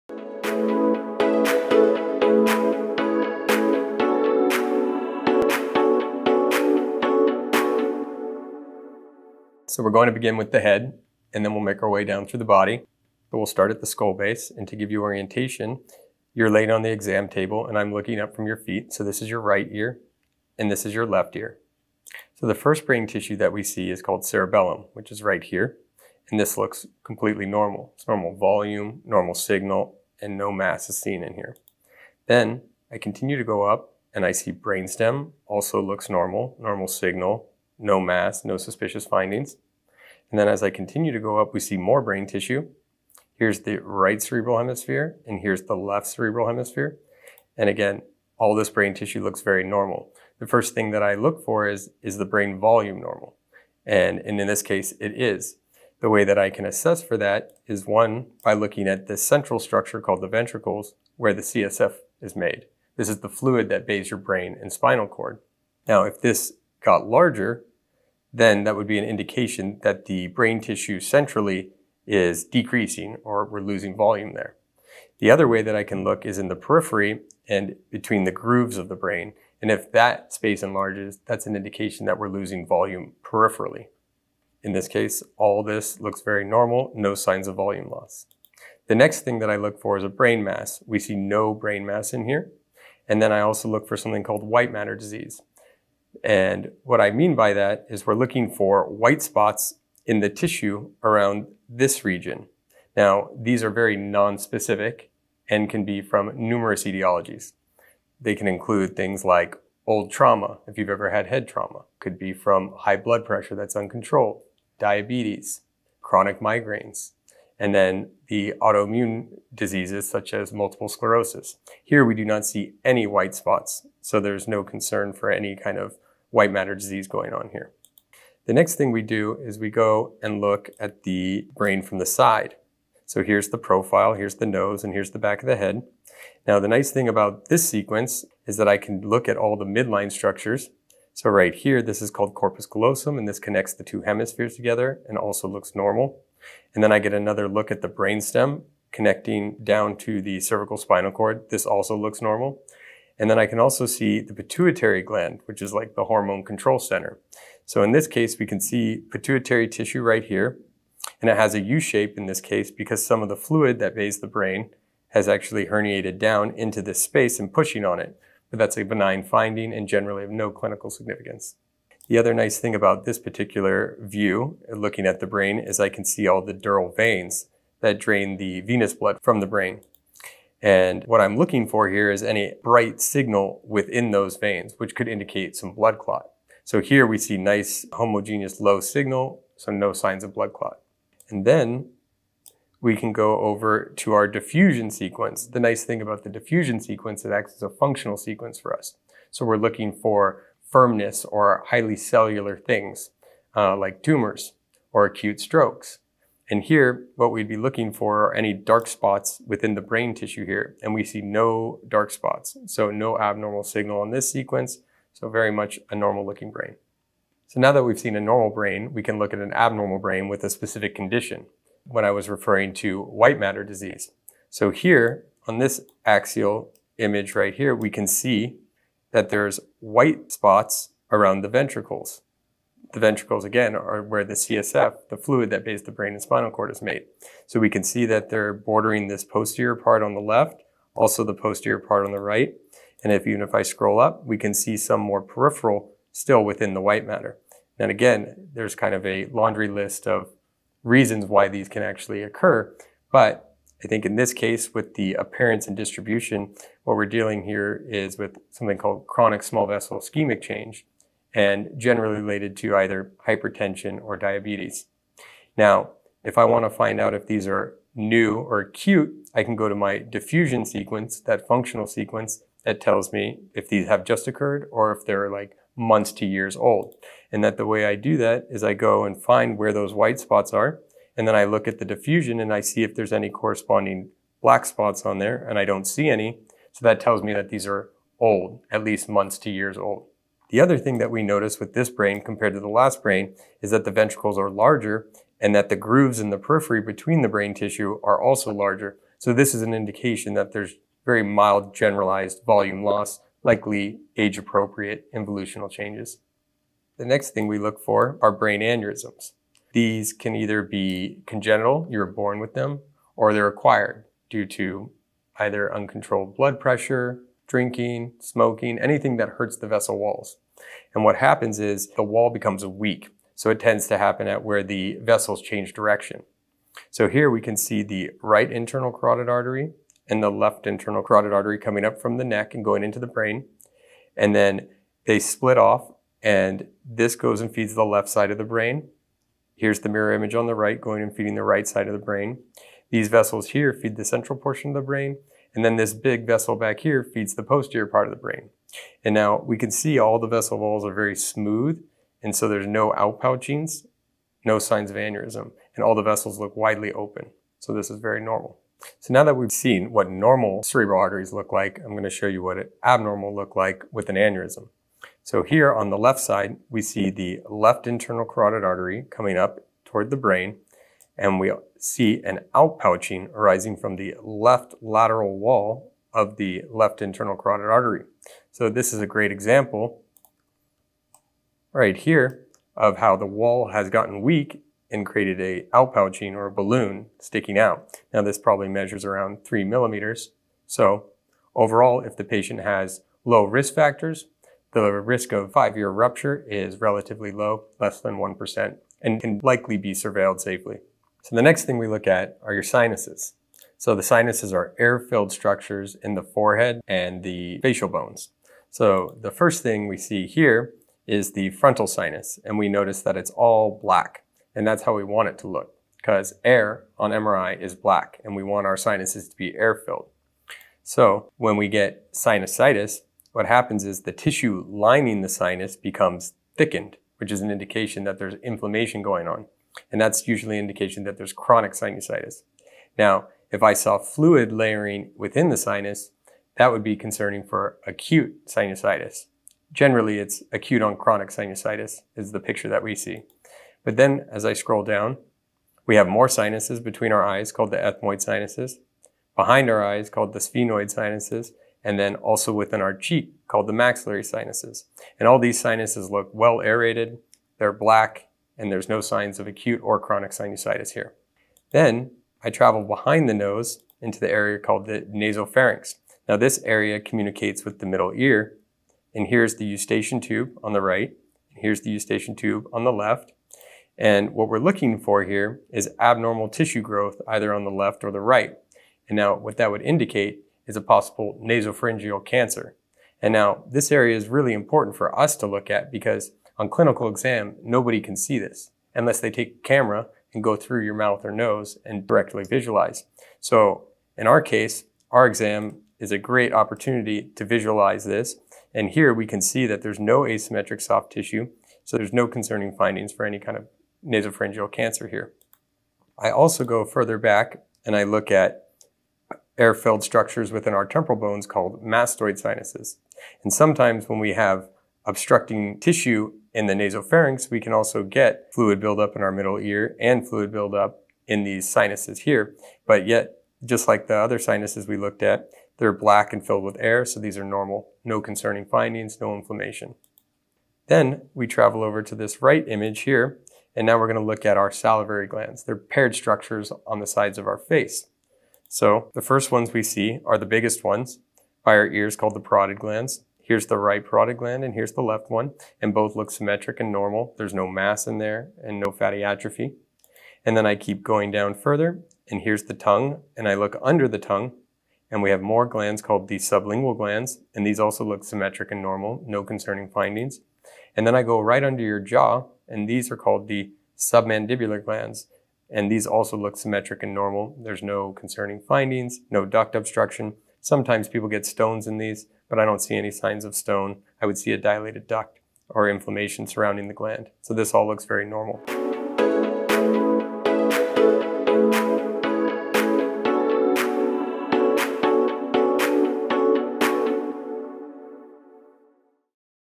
How a Prenuvo MRI Scans the Head | Whole Body MRI Scan
Learn how a Prenuvo scan detects abnormalities in the head and cranial regions and how it can offer fast, safe, and highly detailed radiologist-reviewed reports that scan for over 500 cancers and conditions.